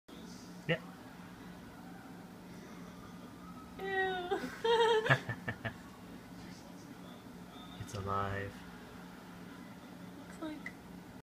Jellyfish shot!
These are fun to make and taste awesome. They also weird people out.
Drinking nights every Saturday!